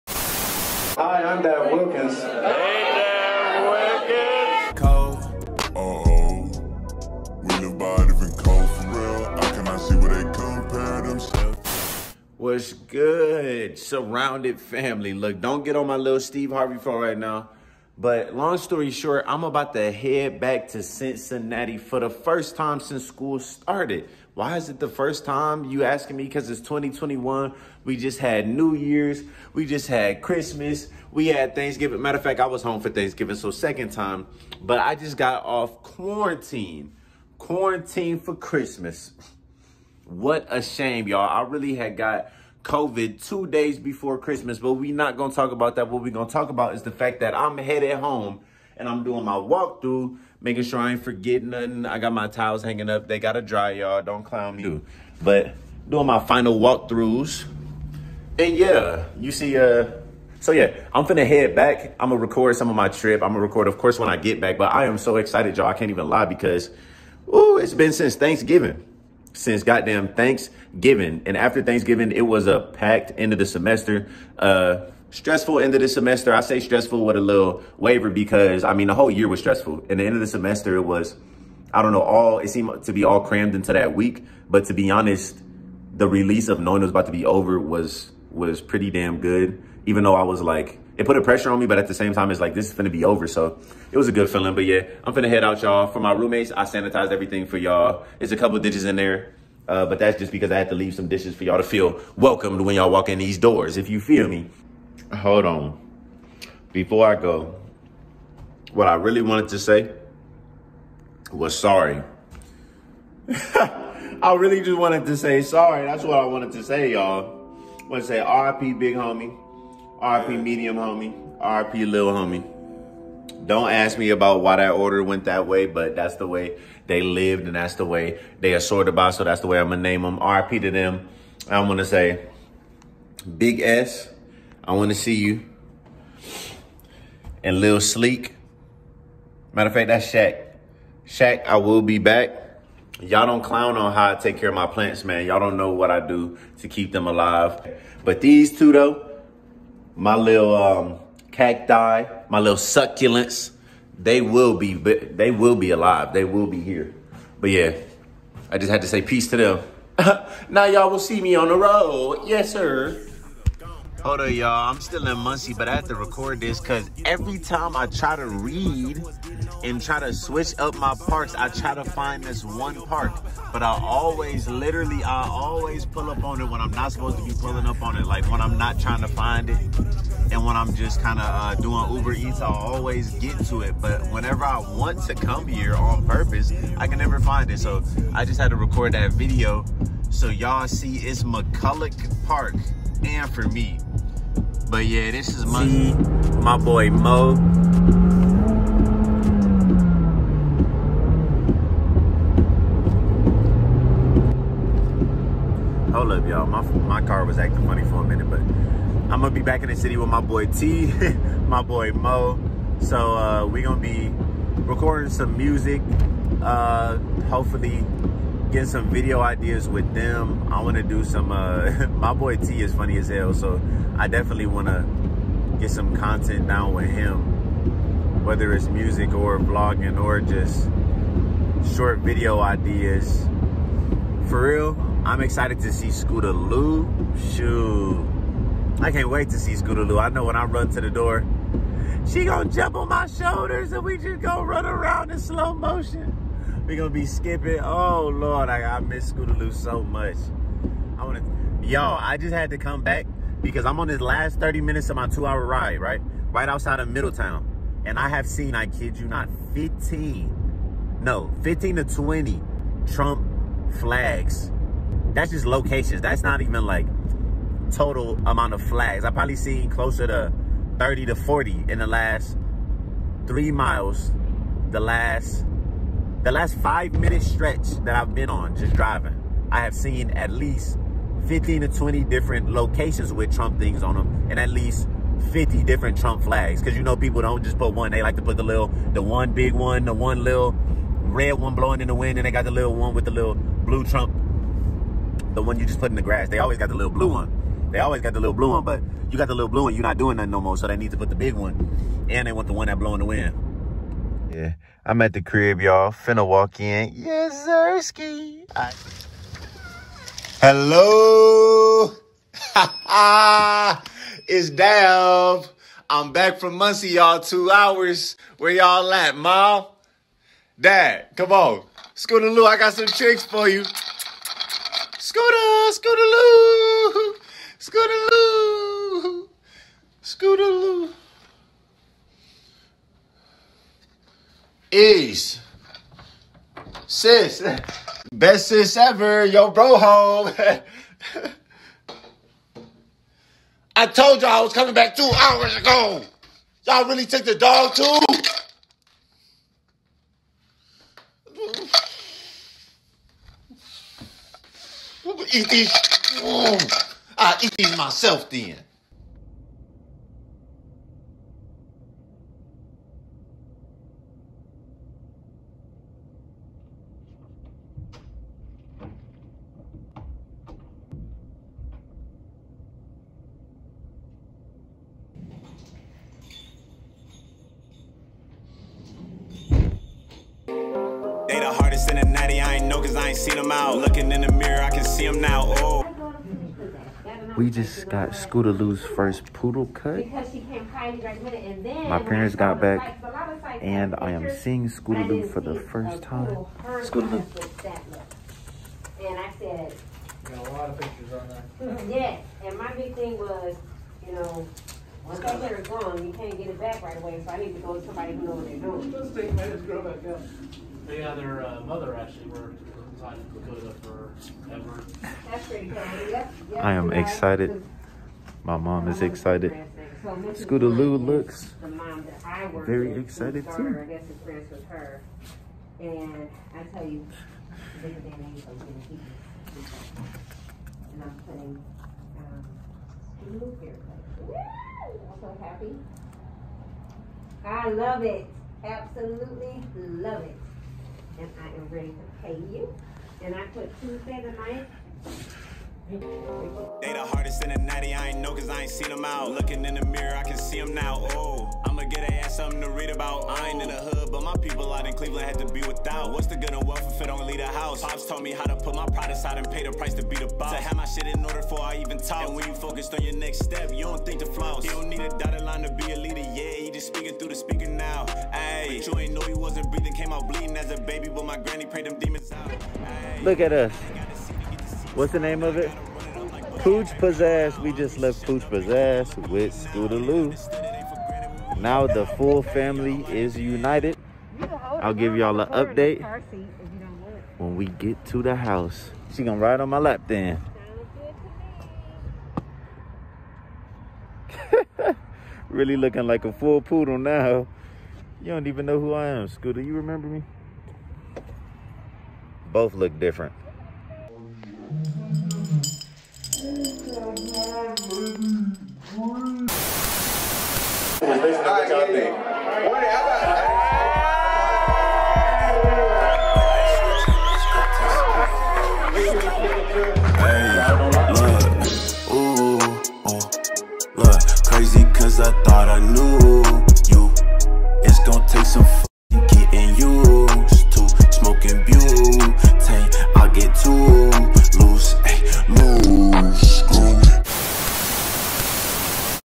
REUNITING WITH MY FAM AFTER SPENDING THE HOLIDAYS QUARANTINED! *Scootalou gets a hair cut*
In this video I finally get to see my fam after spending Christmas and New Years quarantined. Also took my dog to the groom!